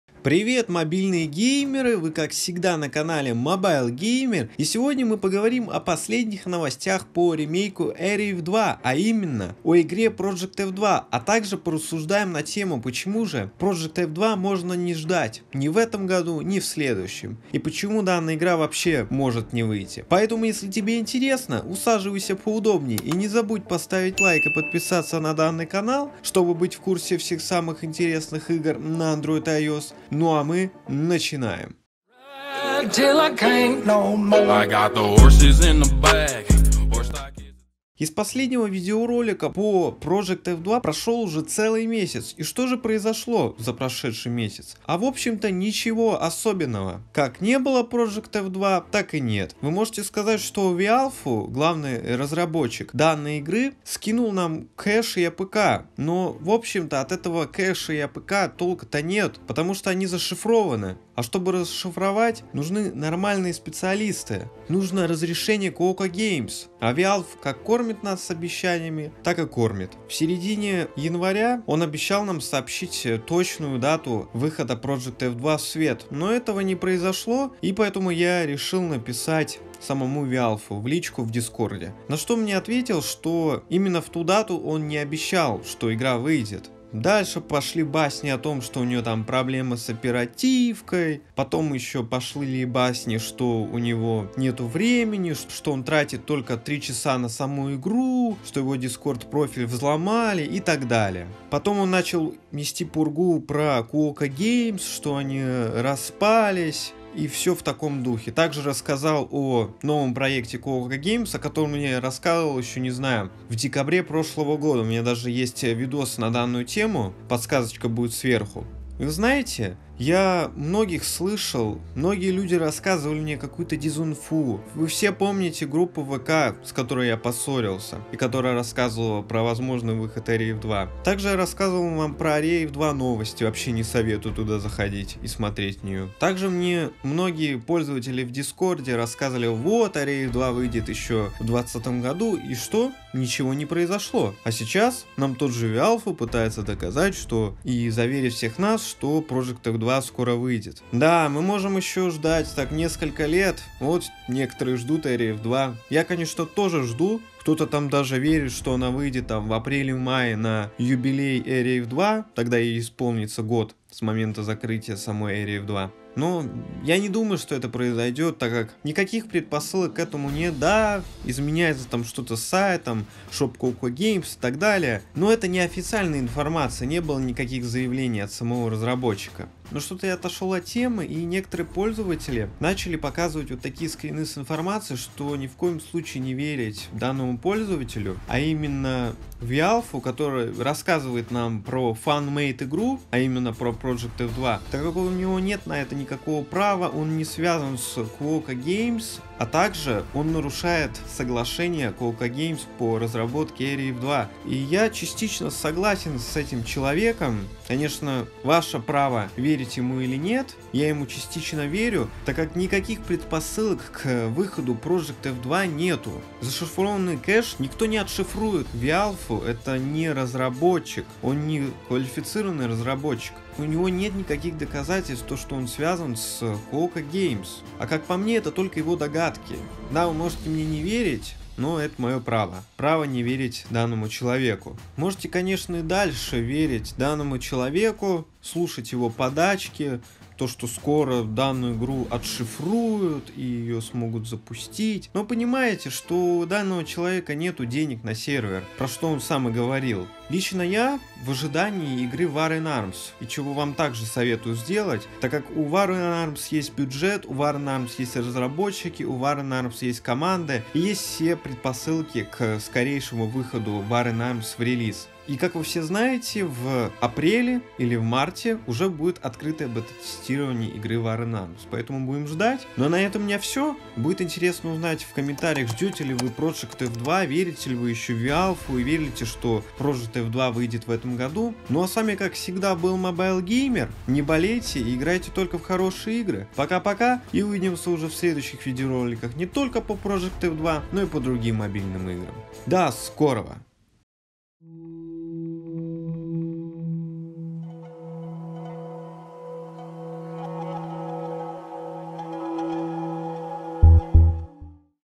PROJECT F2 - ЭТО КОНЕЦ - ПОСЛЕДНИЕ НОВОСТИ ПО РЕМЕЙКУ AREA F2
Привет друг!  В этом видео мы посмотрим и разберём последние новости по неофициальной версии  ареа ф2 - project f2 , так что усаживайтесь поудобней, ну а мы начинаем!

📢Ссылки на источники игры (Project F2)📢